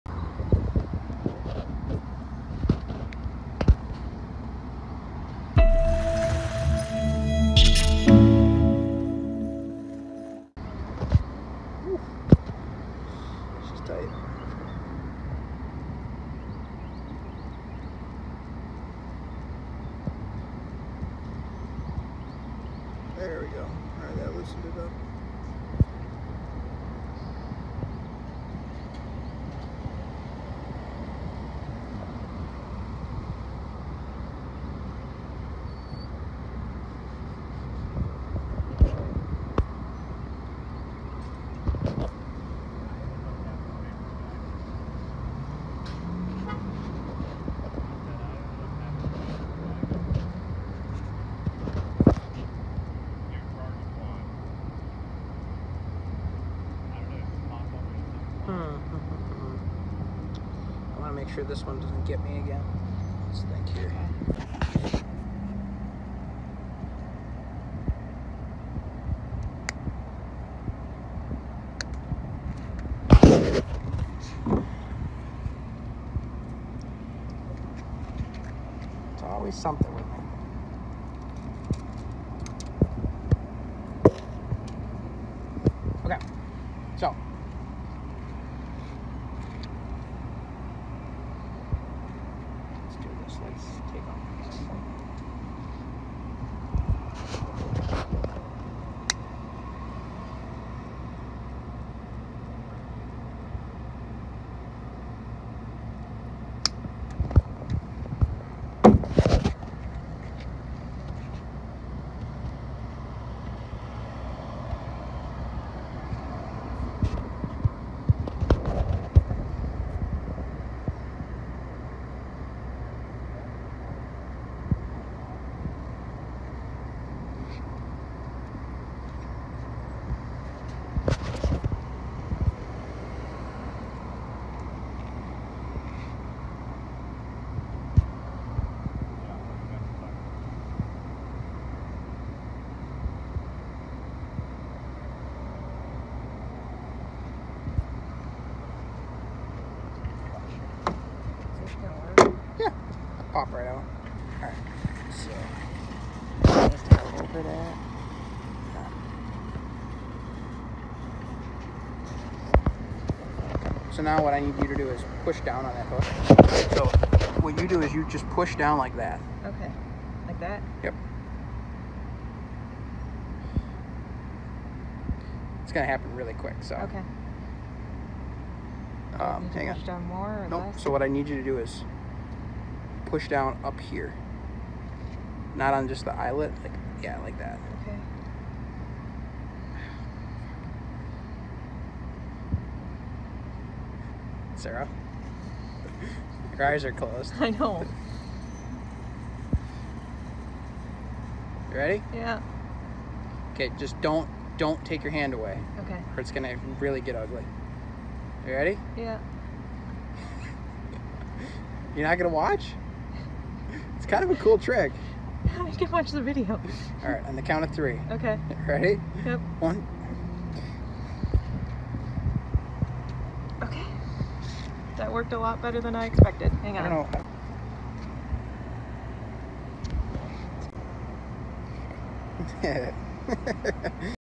How to: Remove a Hook From Your Arm
Kingfish Rig Stuck Me in The Arm While Adjusting Rods in the Back of The Truck